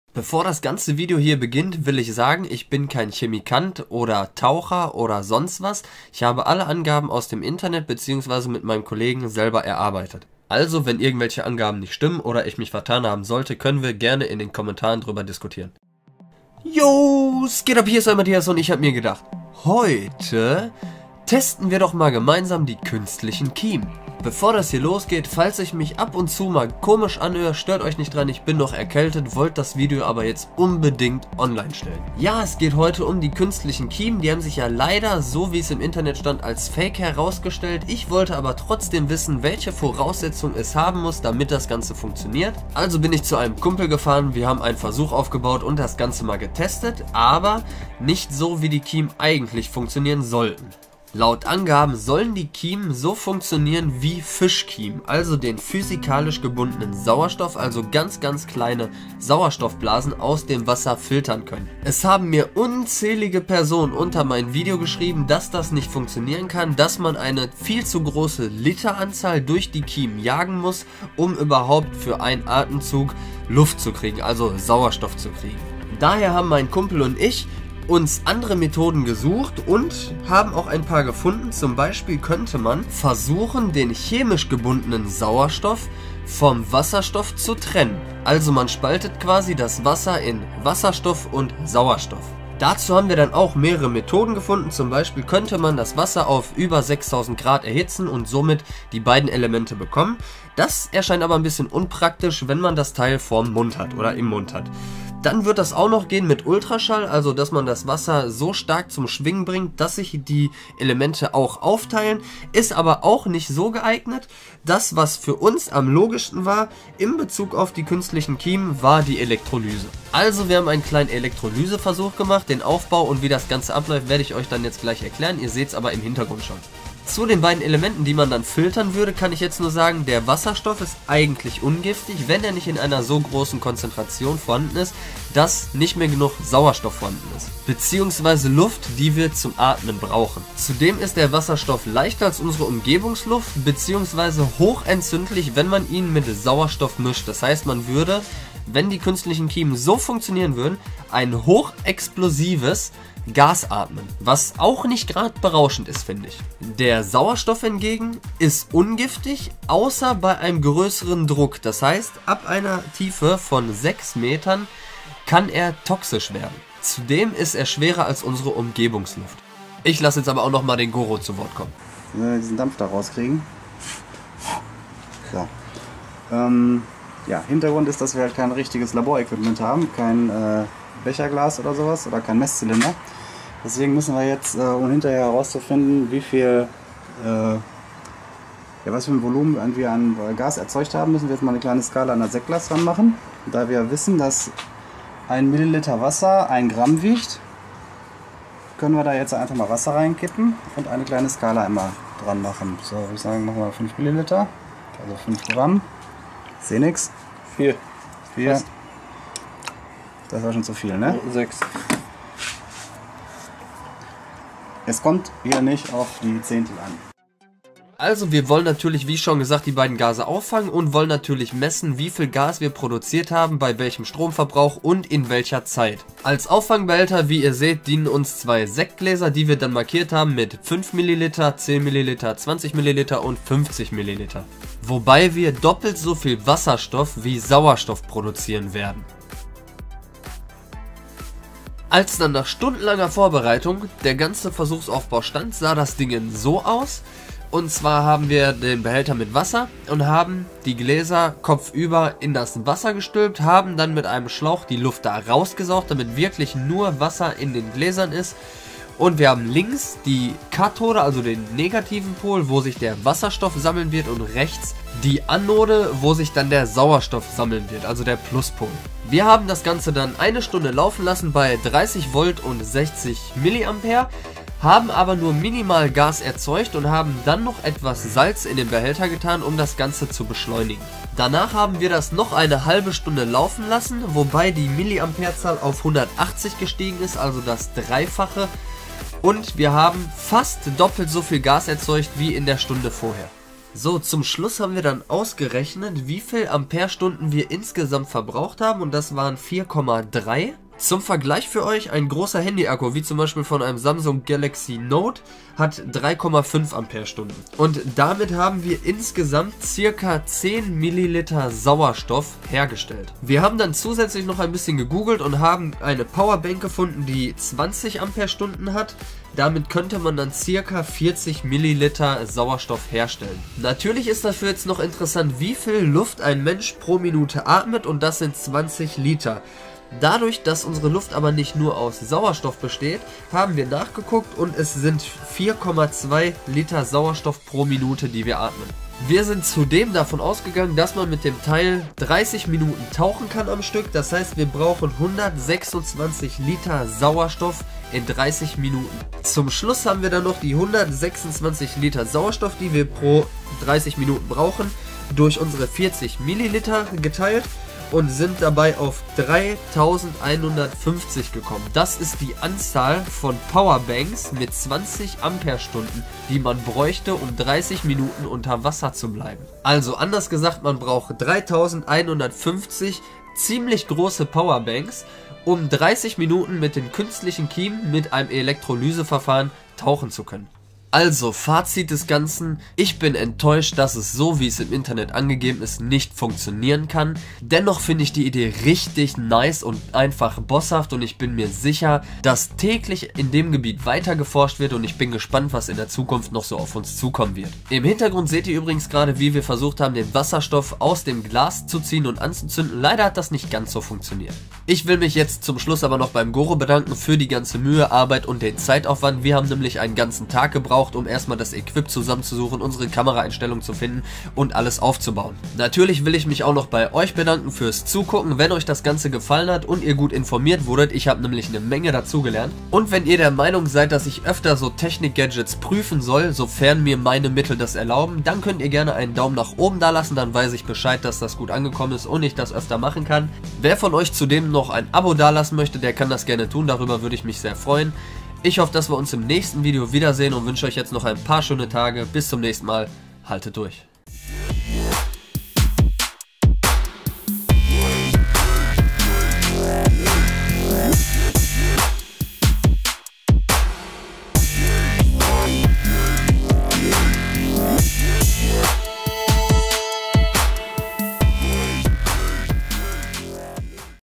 Künstliche Kiemen der Test | Tauchen ohne Flasche geht das? | Kiemen Mythos | Experiment |MLT
Jo Leute,
wie schon lange angekündigt, ist hier endlich das Video "Künstliche Kiemen der Test".
Viele haben sich nach dem ersten Video gefragt: Tauchen ohne Flasche geht das?
Ich habe mit einem Kumpel ein Versuch aufgebaut um die künstlichen Kiemen zu testen und euch eine Antwort zu geben.

Schaut auf meinen anderen Seiten vorbei und bleibt auf dem Laufenden!